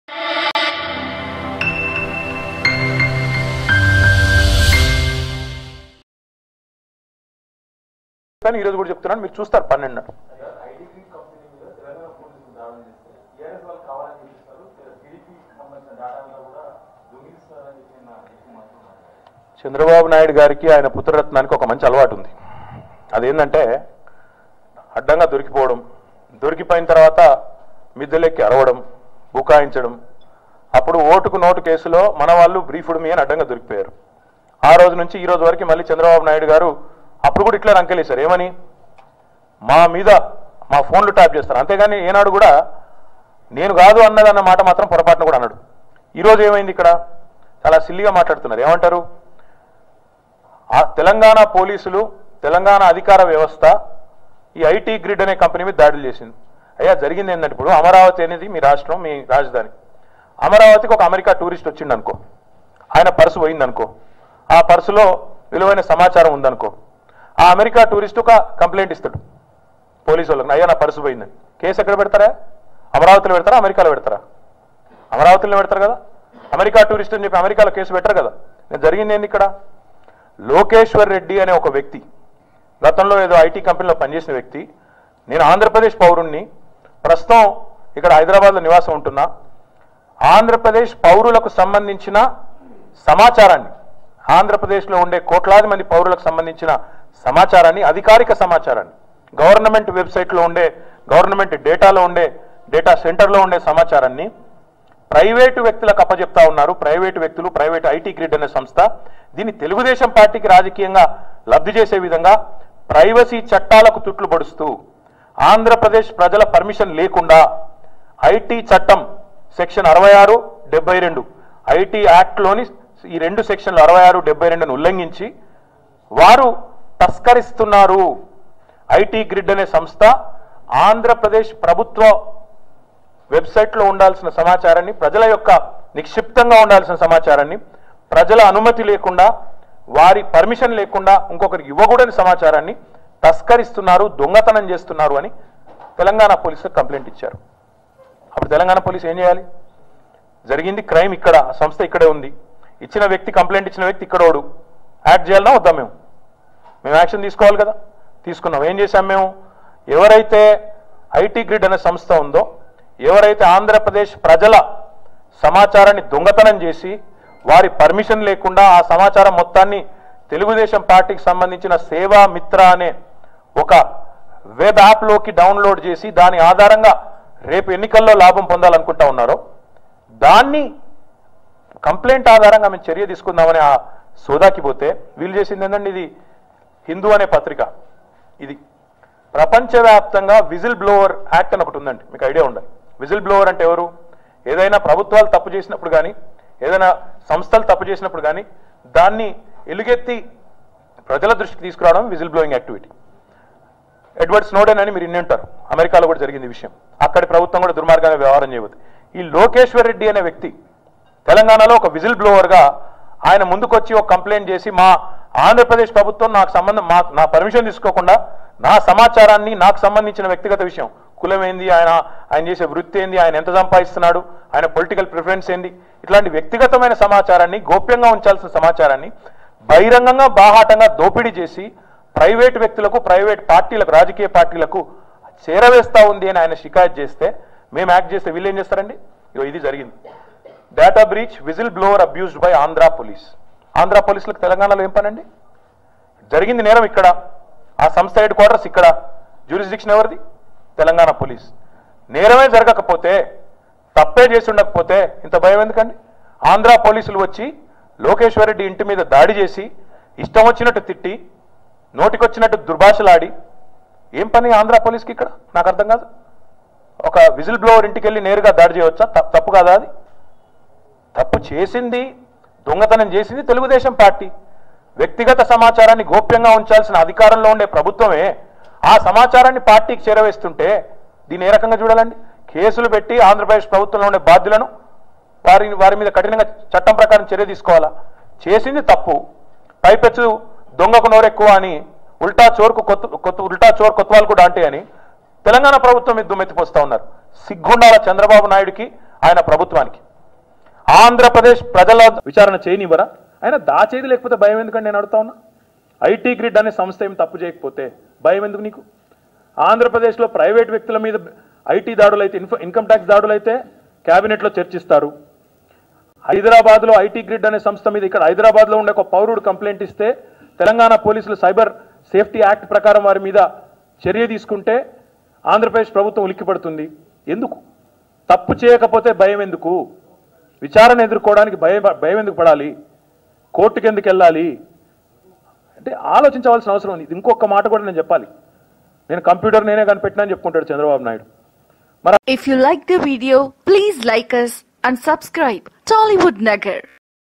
KTR Fires on Chandrababu Naidu and Nara Lokesh Over Data Theft Case IT Grid | TRS | Tollywood Nagar
Hey guys!